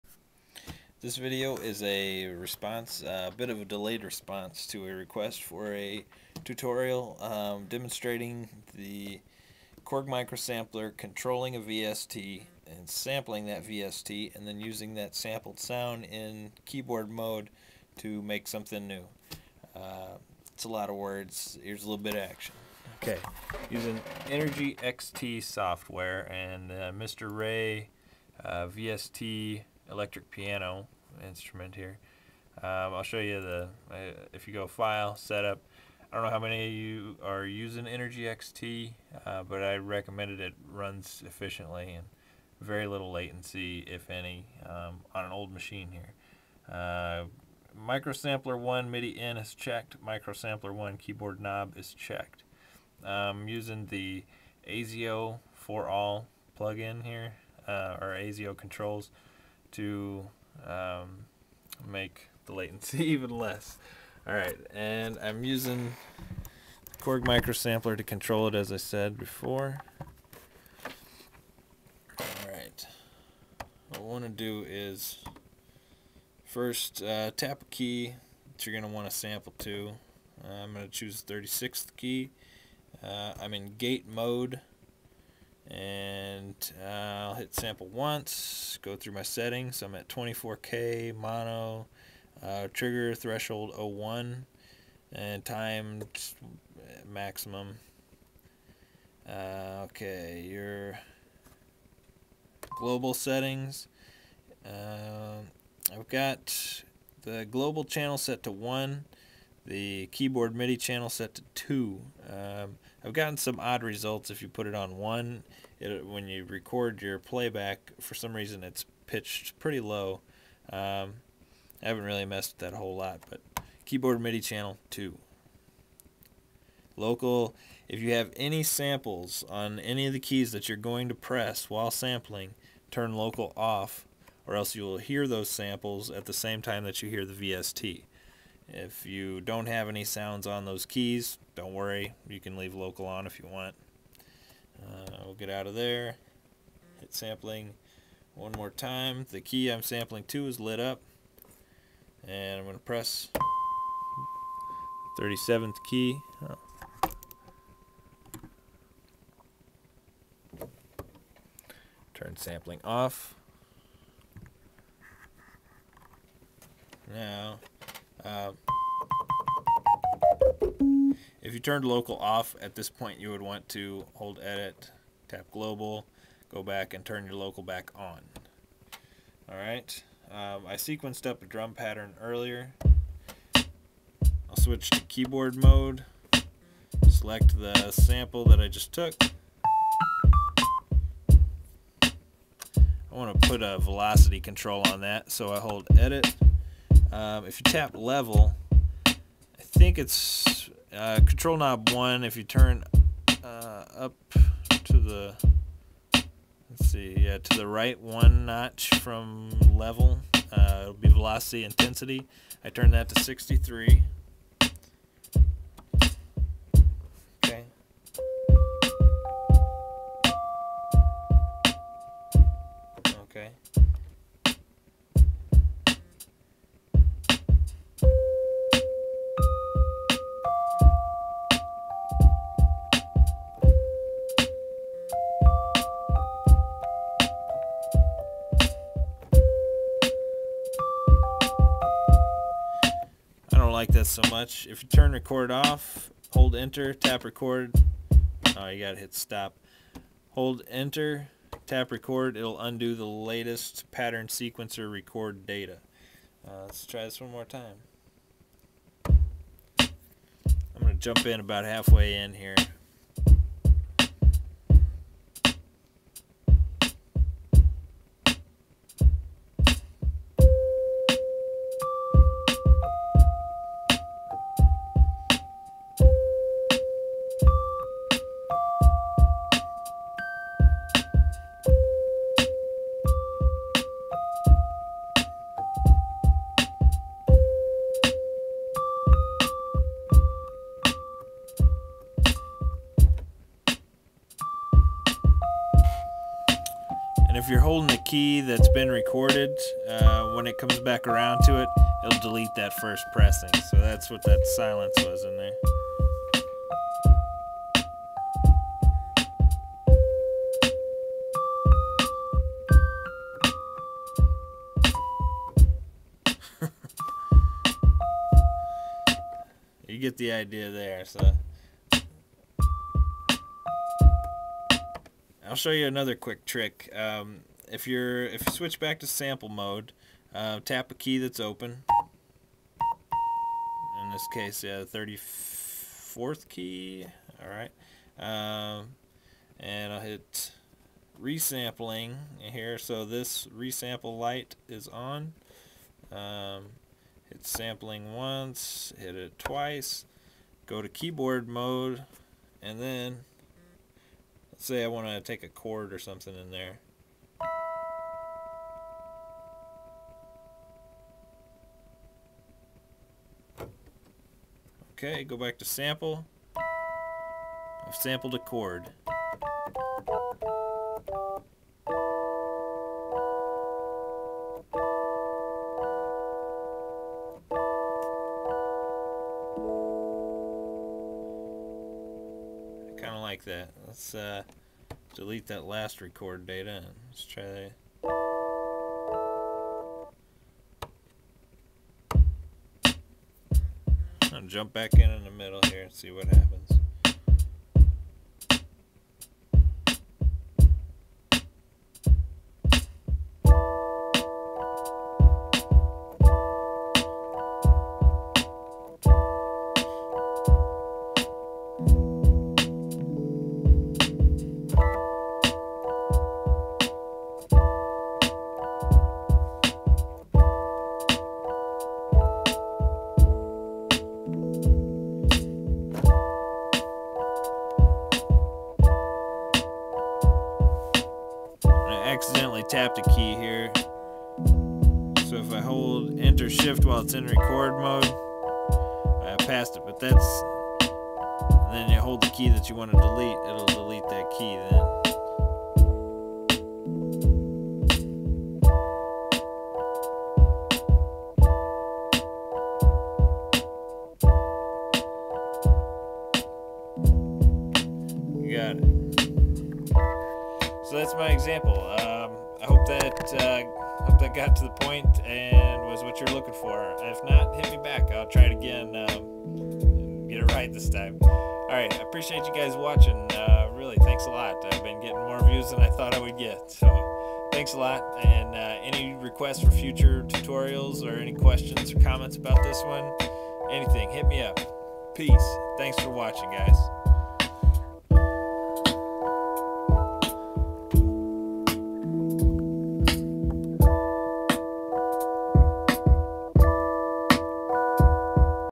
KORG microSAMPLER + Energy XT = VST controlling and sampling tutorial
This one is a bit of a delayed response to a request for a tutorial on sampling a VST and using that sample in Keyboard mode on the MS-1. I hope this is helpful in some way. I'm using Energy XT and the Mr. Ray '73 VST instrument on a Windows XP machine and sampling directly into the MS from the computer line out. If you have any questions or requests for future tutorials, please feel free to drop me a line. Peace.